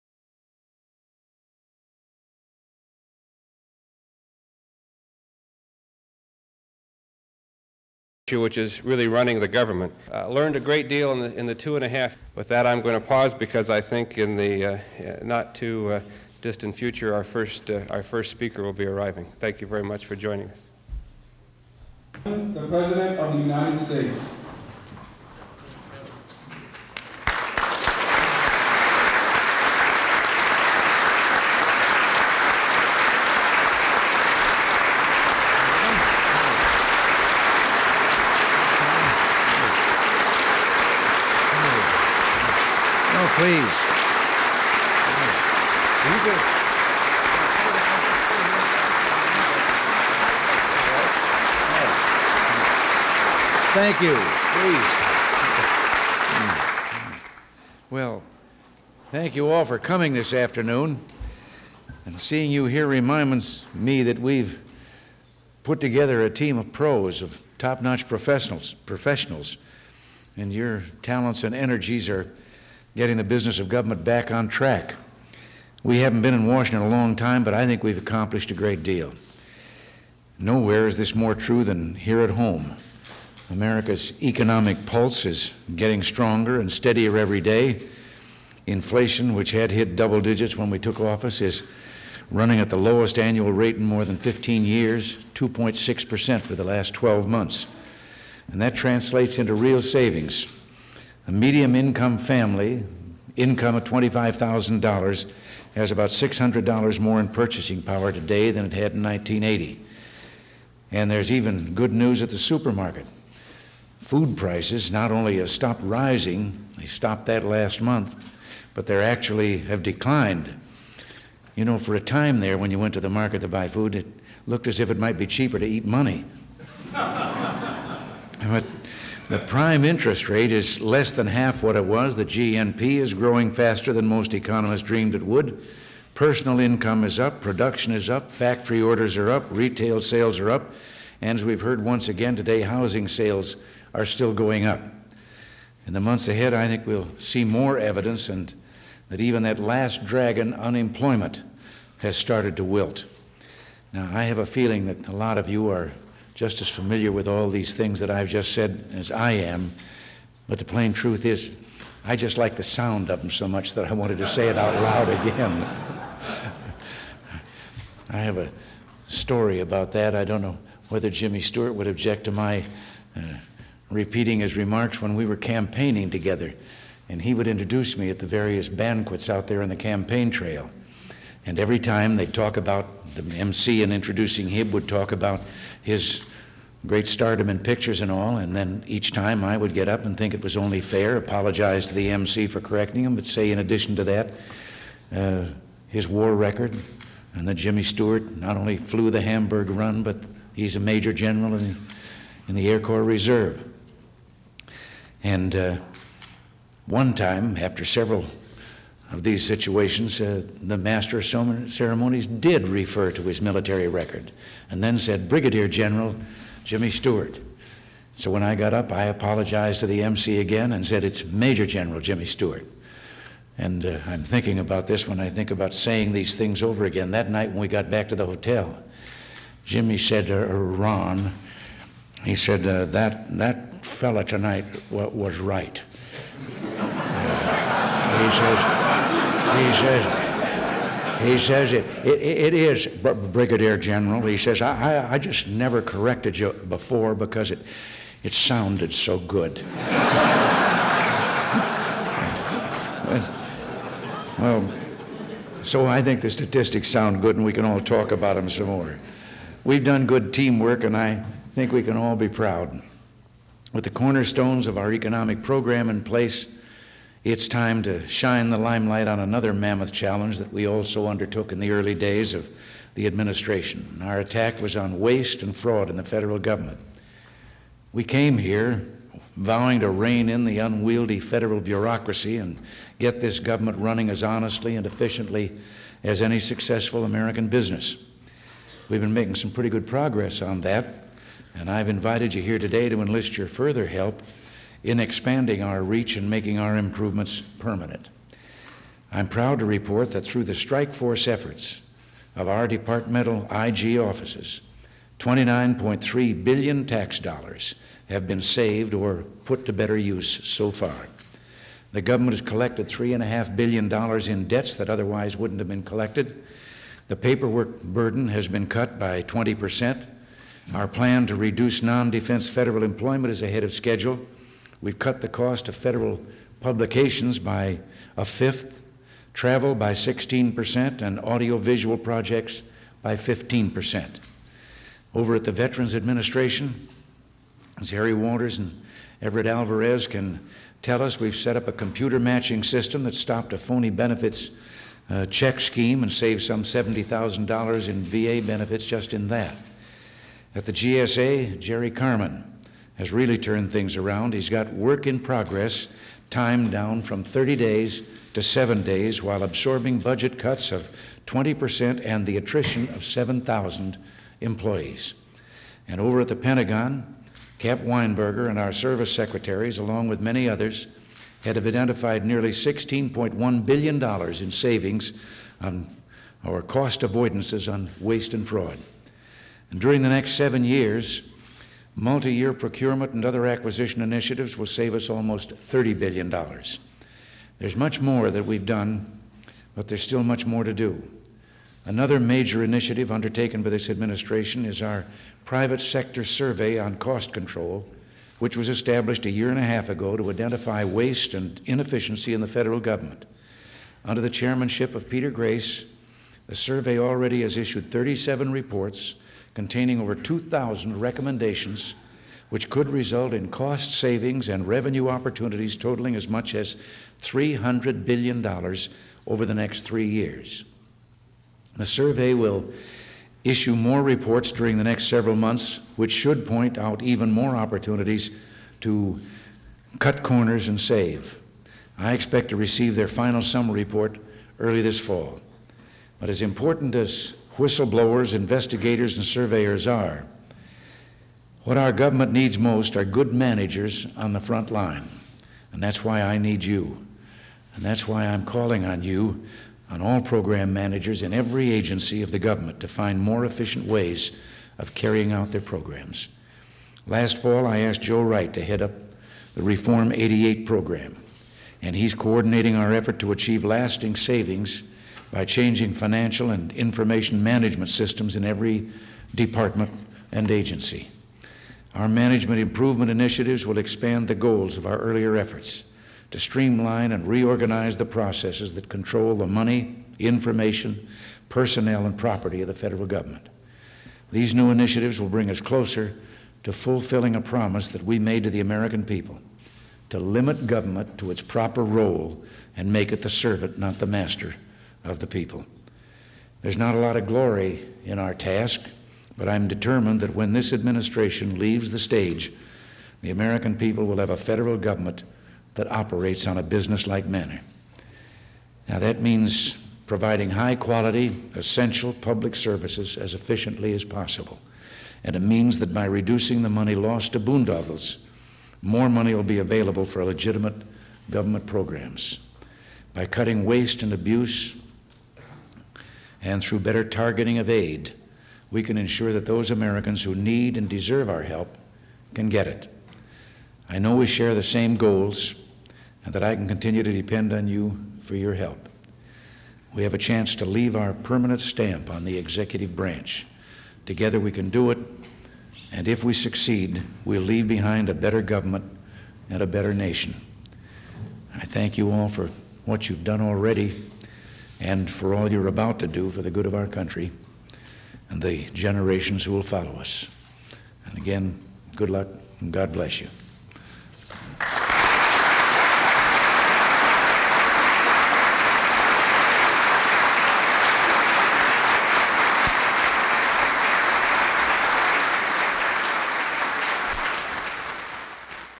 President Reagan's Remarks at a Briefing on Federal Management Reform on August 2, 1983
Full Title: President Reagan's Remarks at a White House Briefing for Administration Officials on Federal Management Reform in the East Room on August 2, 1983

Access Restriction(s):Unrestricted
Use Restriction(s):Unrestricted